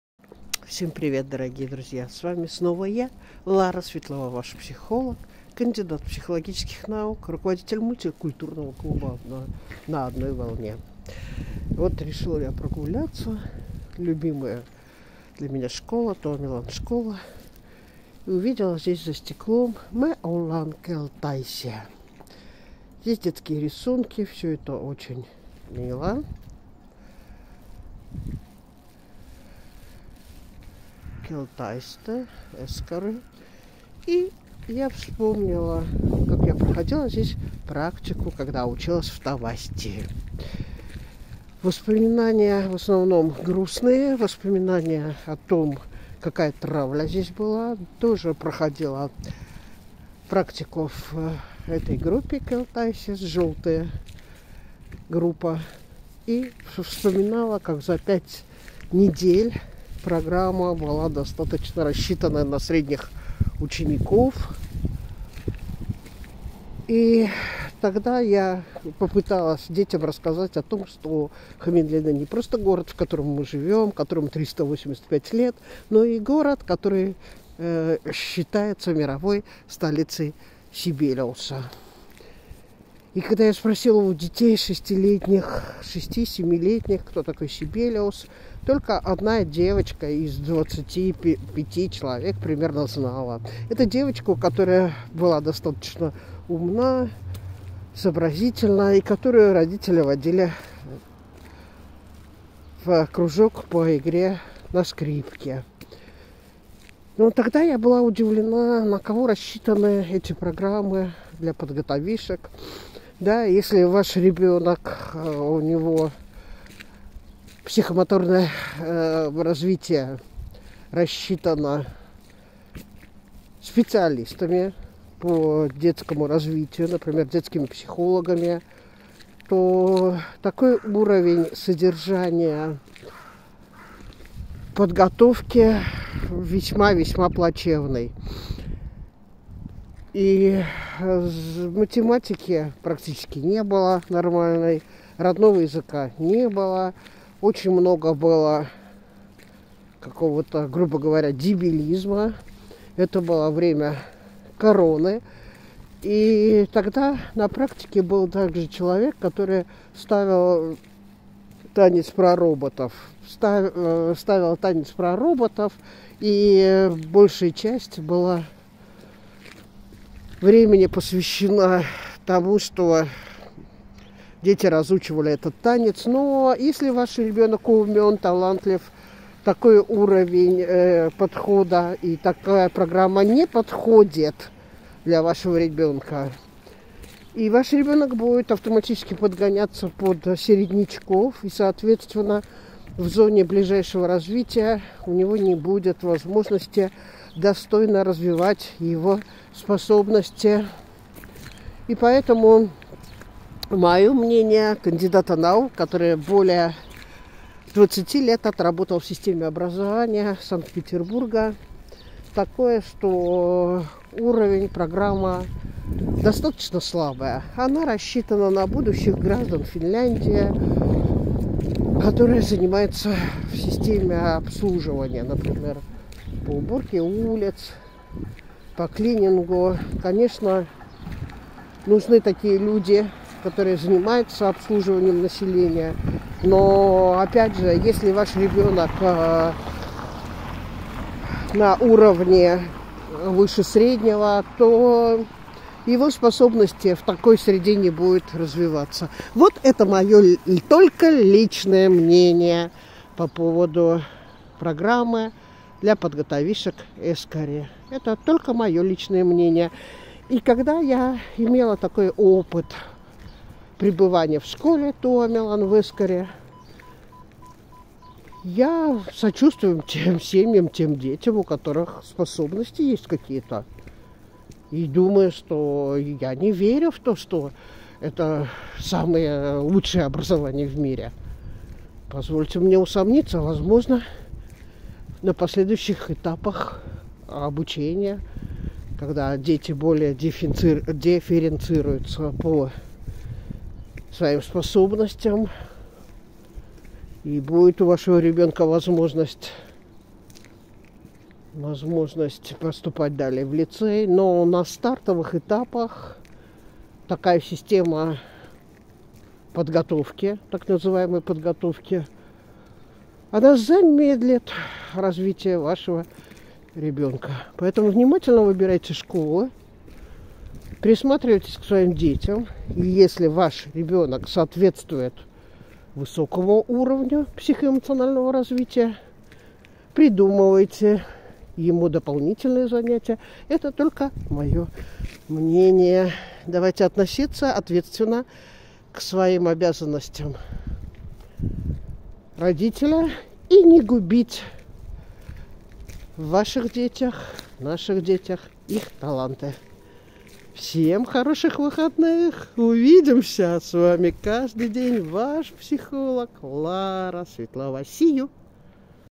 my thoughts on the program for preparing children for school at Tuomelan School
Людмила : Продолжаем изучать английский язык  ( урок 8) в клубе

а Финляндии якобы лучшее образование. в Финляндии якобы живут самые счастливые люди мира. позвольте мне как специалисту педагогу- психологу высшей категории усомниться вновь в этом.